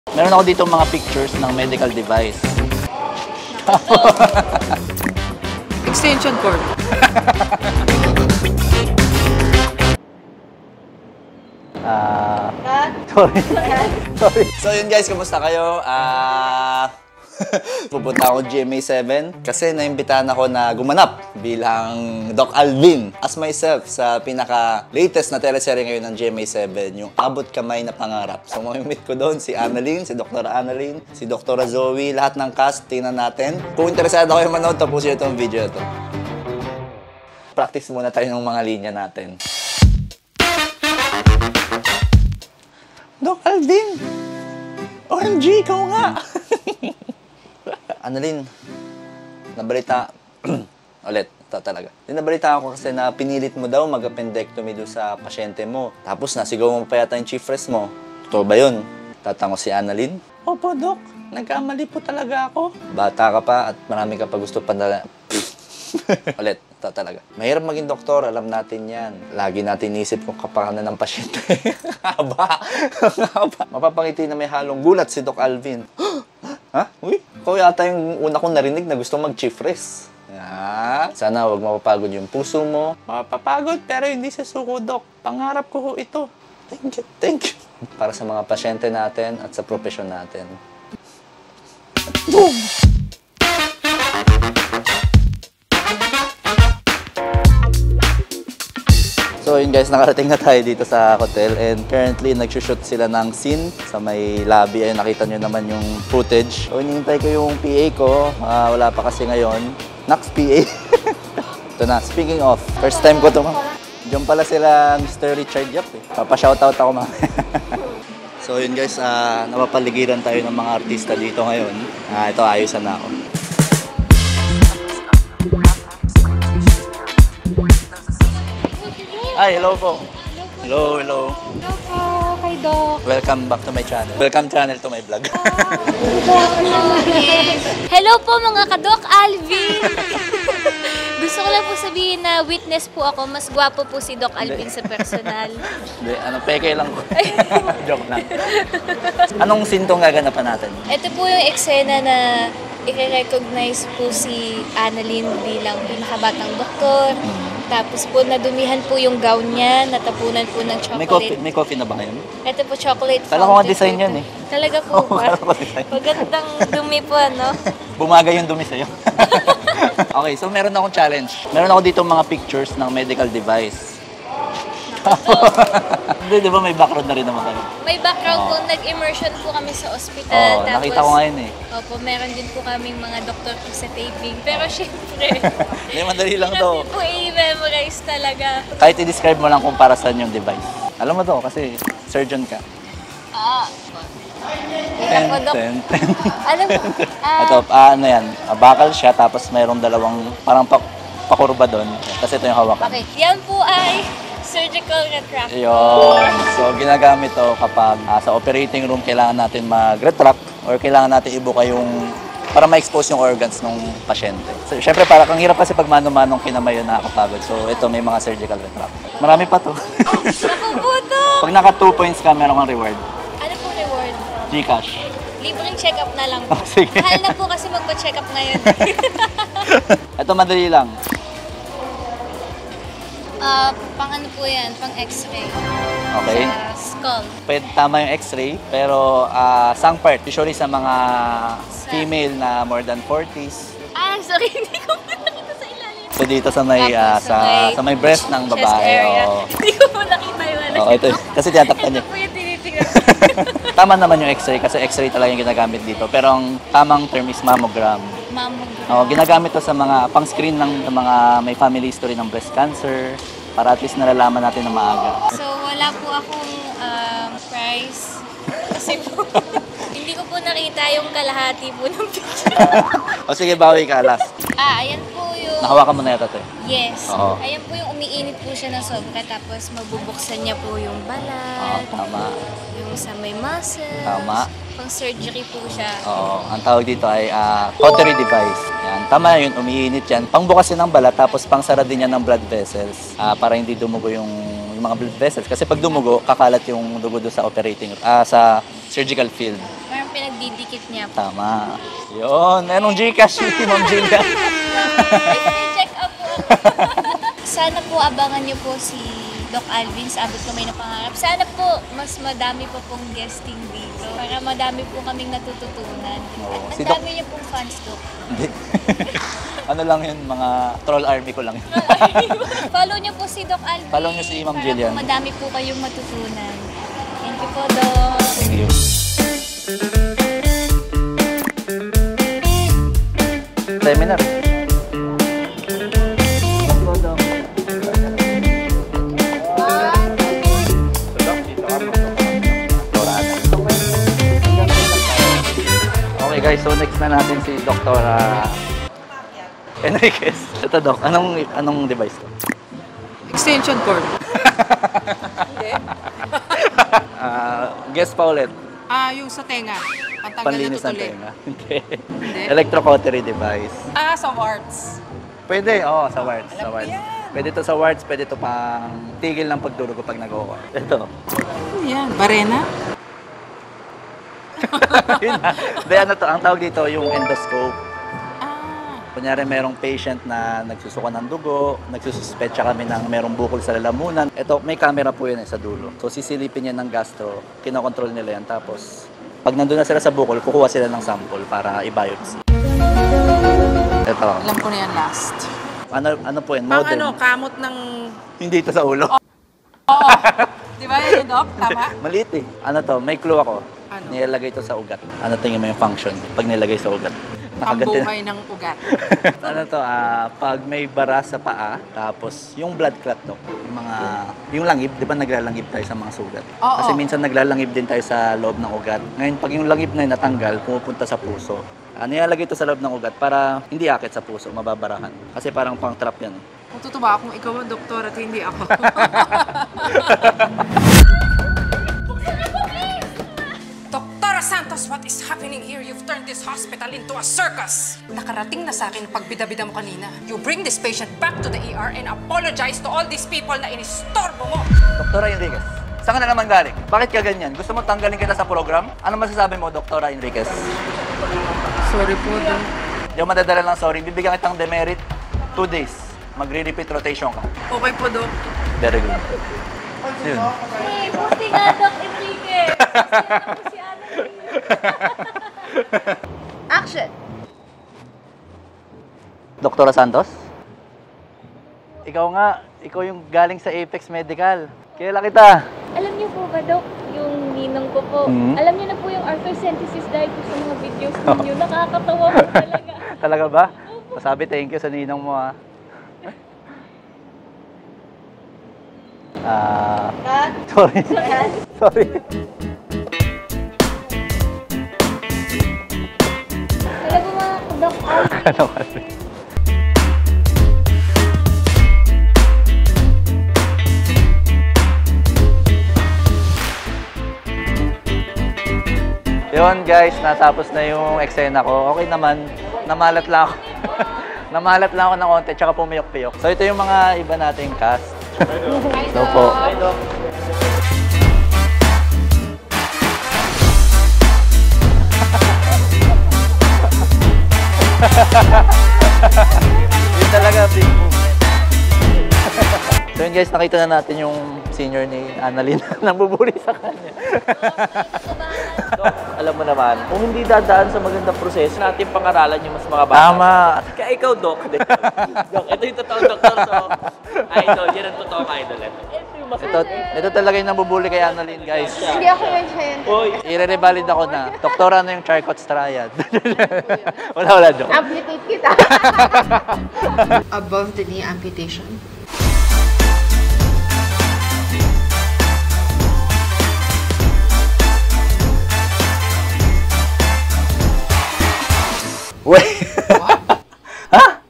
REAL DOCTOR V.S. PINOY MEDICAL DRAMA CASTS
Nirevalida ko si Dra. Enriquez at Dr. Pardo